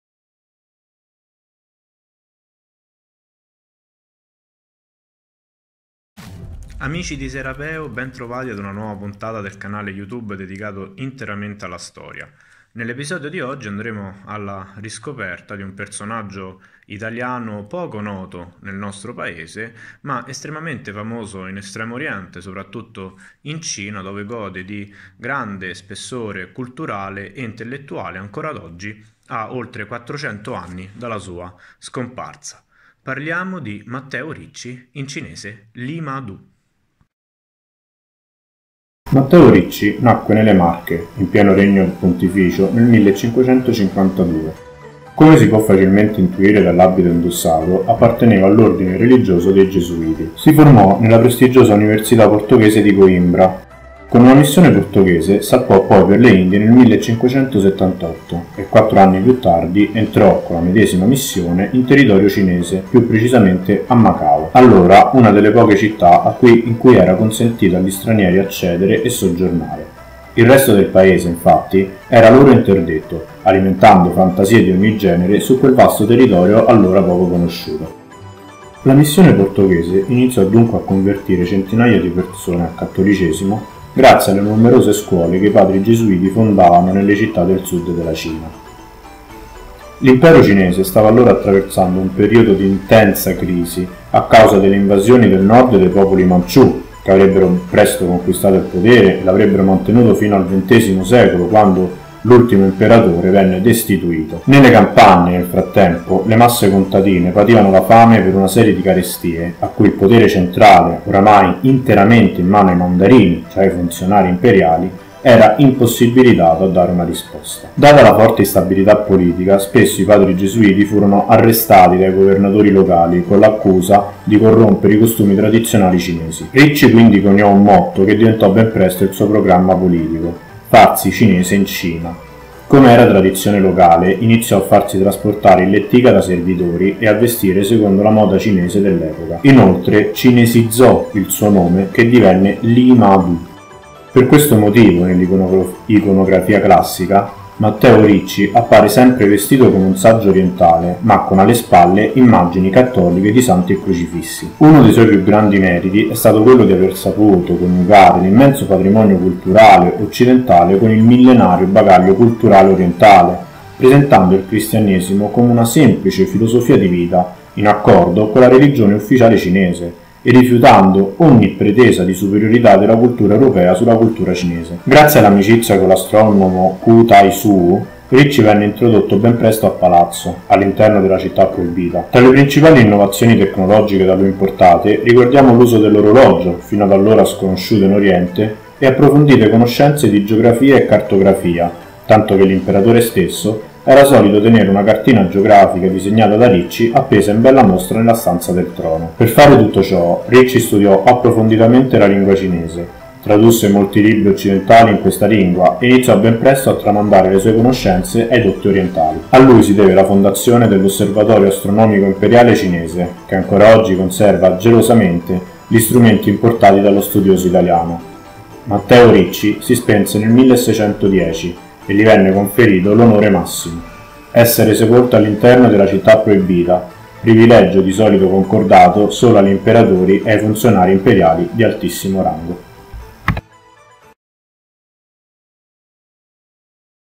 Alla scoperta della Cina: Matteo Ricci, in arte Li Ma Du
Matteo Ricci, gesuita italiano del XVI secolo, fu il primo uomo occidentale, dopo Marco Polo, a soggiornare stabilmente in Cina. Uomo di cultura e di scienza, seppe scalare le gerarchie della corte imperiale diventando uno dei sapienti più influenti nel regno. Ancora oggi, le innovazione tecniche da lui importate, sono custodite gelosamente e rappresentano una straordinaria testimonianza dell'incontro tra due millenarie culture